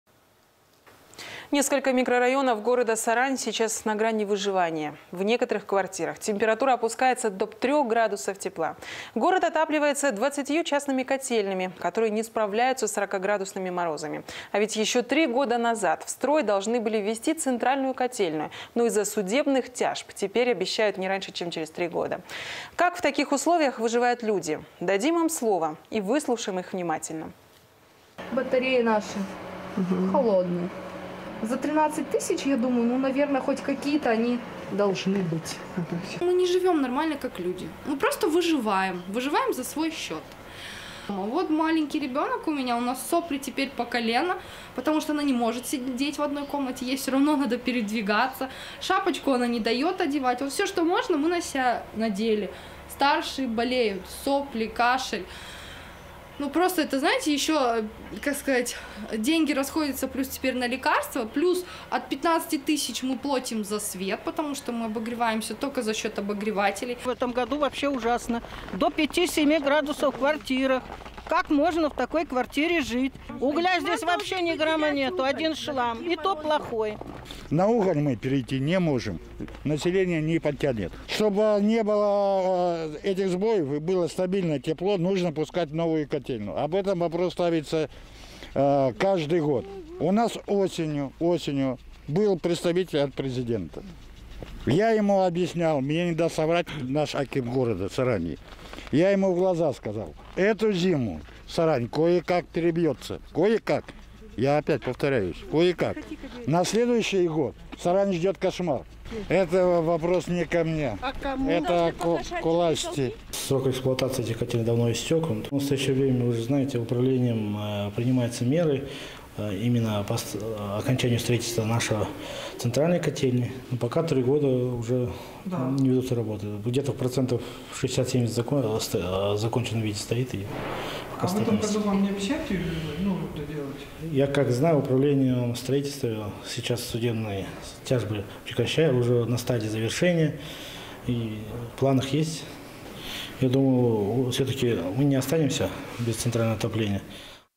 Несколько микрорайонов города Сарань на грани выживания
Несколько микрорайонов города Сарань на грани выживания. В некоторых квартирах температура опускается до трех градусов тепла. Город отапливается 20 частными котельными, которые не справляются с 40-градусными морозами. А ведь еще три года назад в строй должны были ввести центральную котельную, но из-за судебных тяжб теперь ее обещают не раньше, чем через три года.  Как в таких условиях выживают люди?